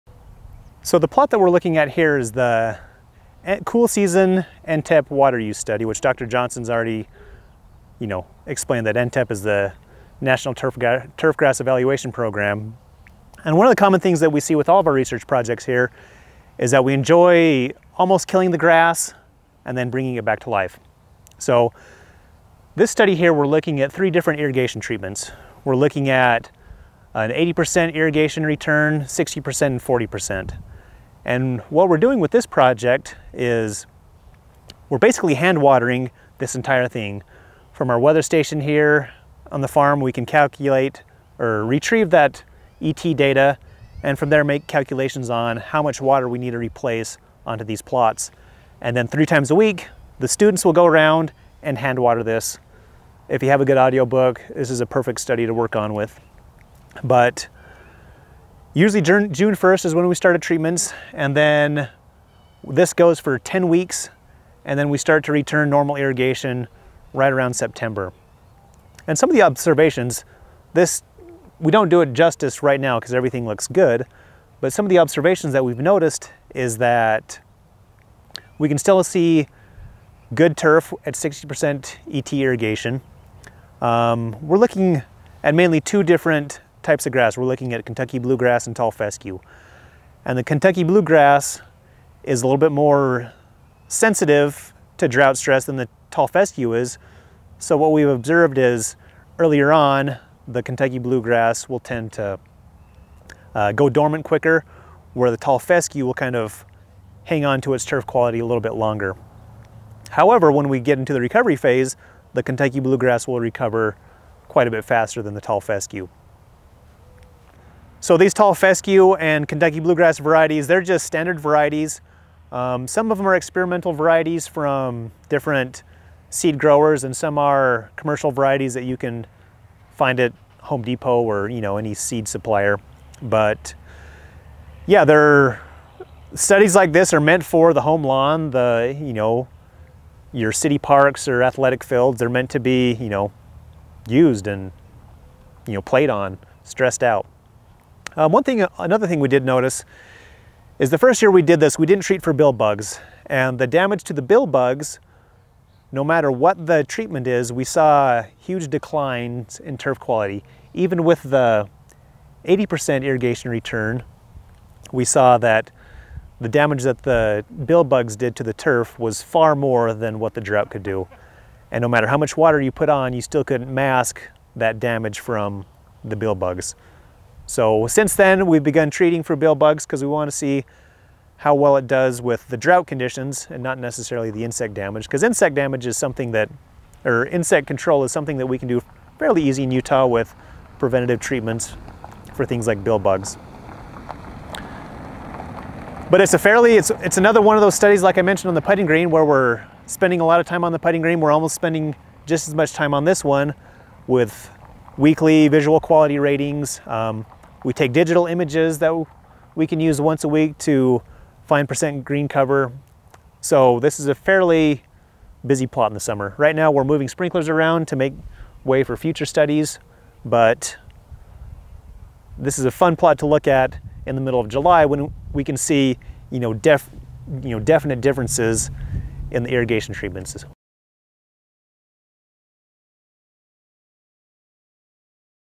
Irrigation Trials for Tall Fescue and Kentucky Bluegrass
Turf irrigation trials conducted at Greenville Research Farm in Logan, Utah.